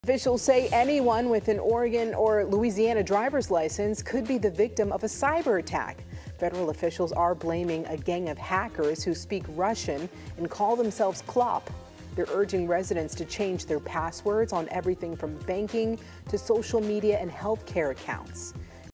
Cyberattack hits Louisiana and Oregon
Louisiana state officials say MOVEit - which is a data transfer service used to send large files - was targeted by cyber attackers.